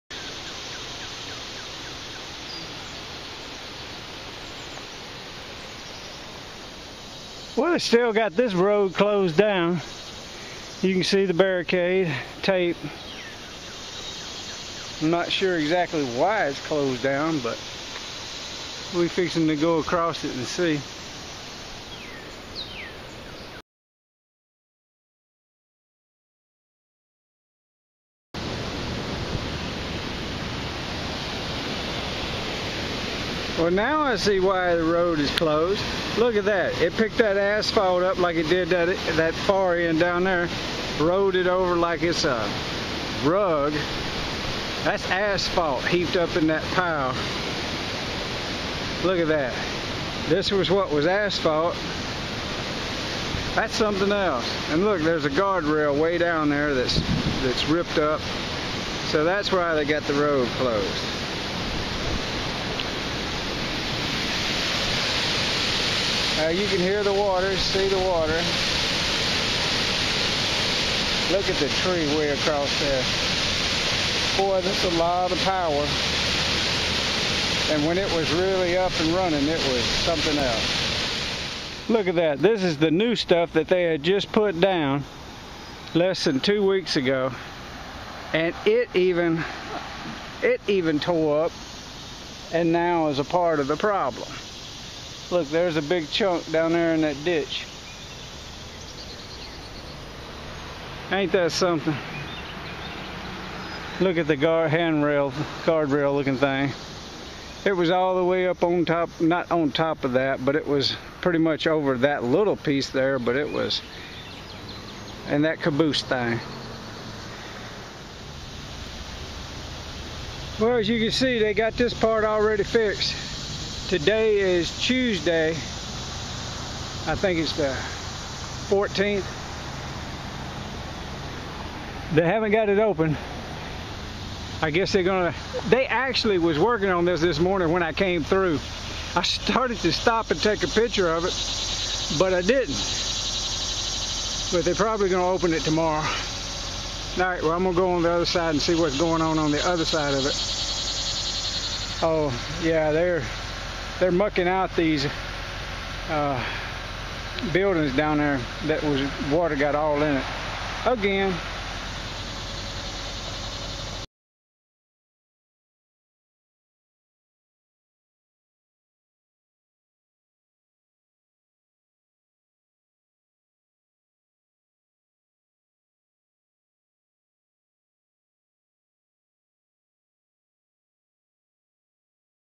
Two Century-old bridge near Ozark, Missouri needs repairs
Two-span through truss bridge over Finley Creek on W. McCracken Street (Old US 65) in Ozark
Now is 07-14-15  Open to traffic
Built 1922 by the Pioneer Construction Co.
Riverside Bridge   [Map · Street View]
Two-span Pratt through truss bridge over Finley Creek on Riverside Drive north of Ozark in Ozark
NOT Open to traffic as of 07-14-15
Built 1909 by the Canton Bridge Co. at Ozark Mill and later relocated here; reopened Aug. 23, 2013